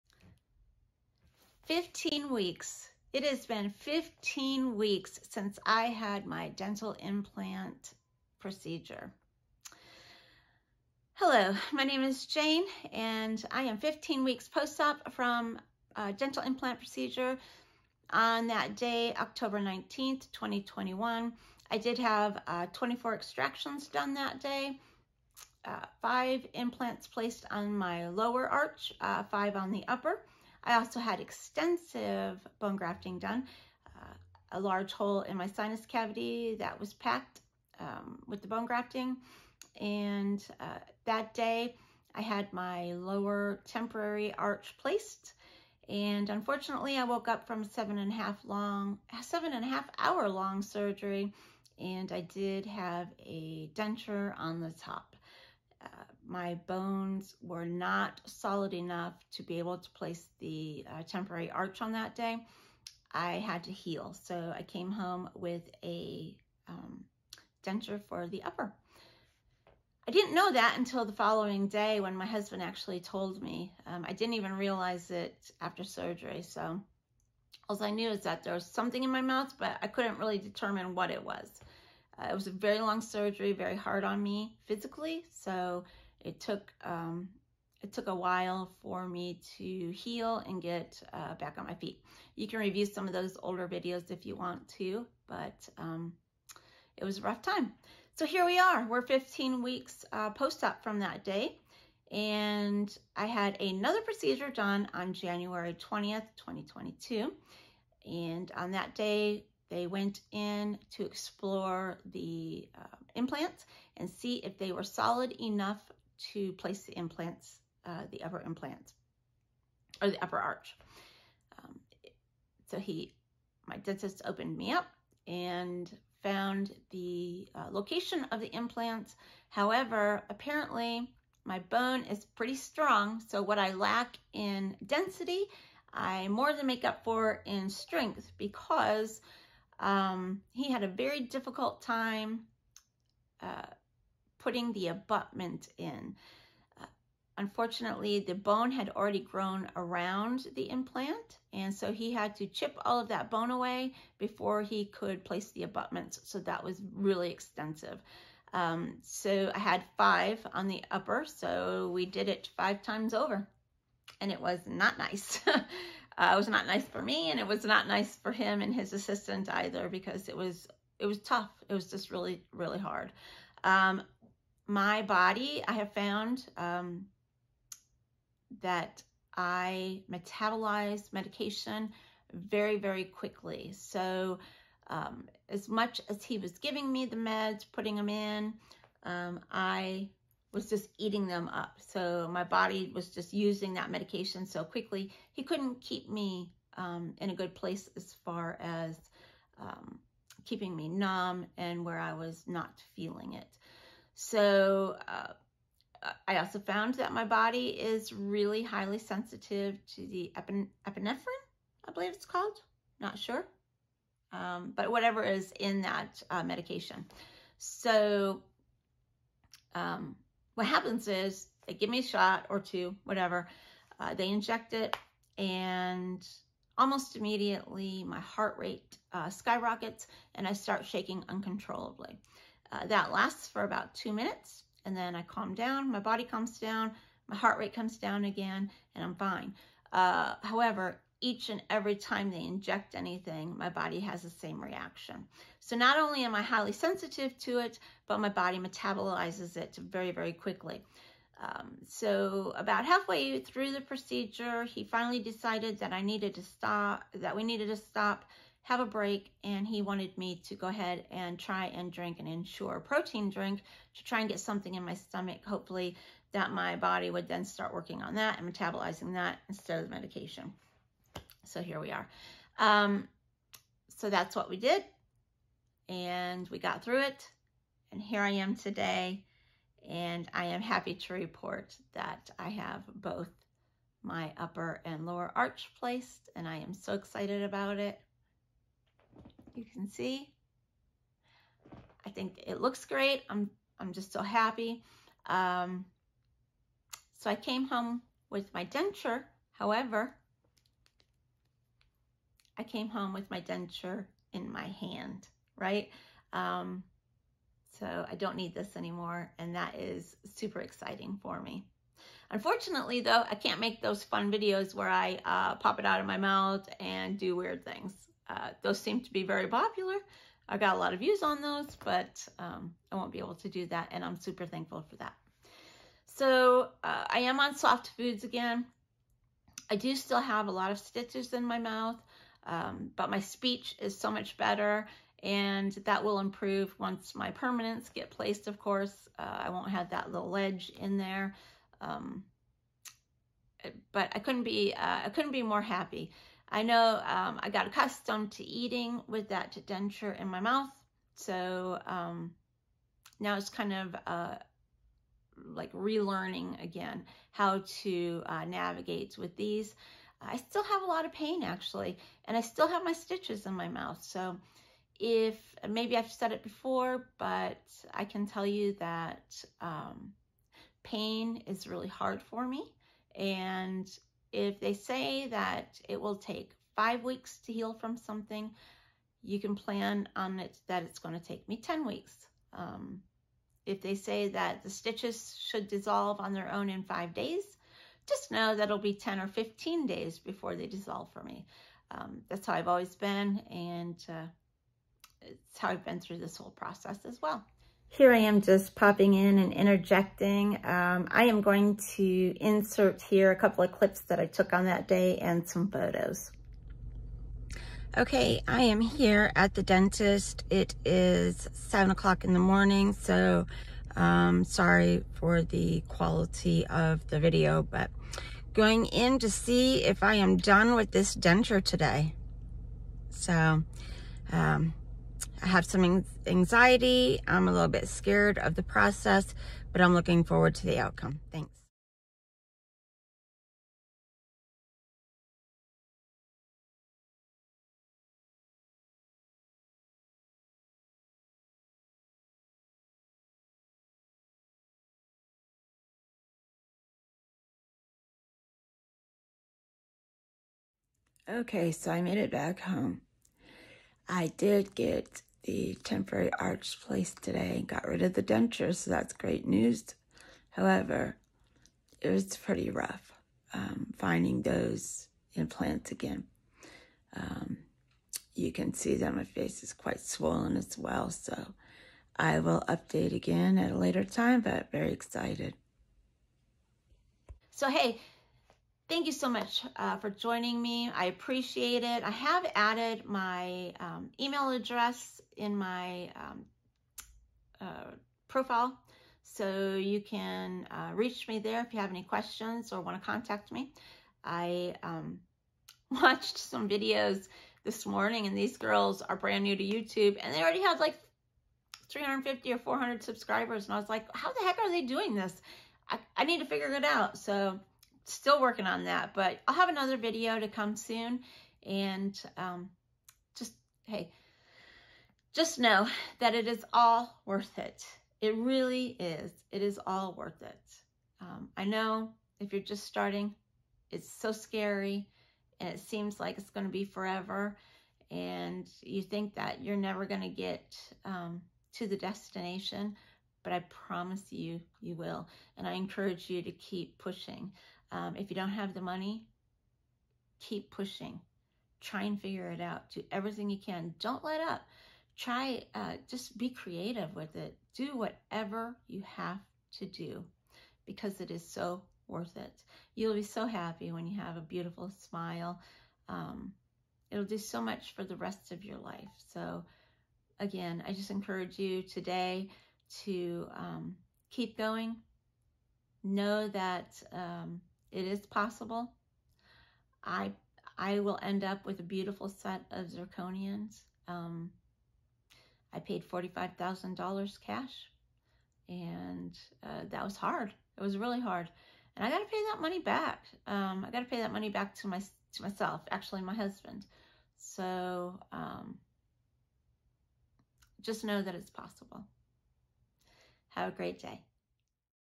15 Week Post Op All on Five (all on four) Dental Implant Surgery. Do I still have a denture????
This is my 15 week post op update. I had a 2nd surgery and share what happened.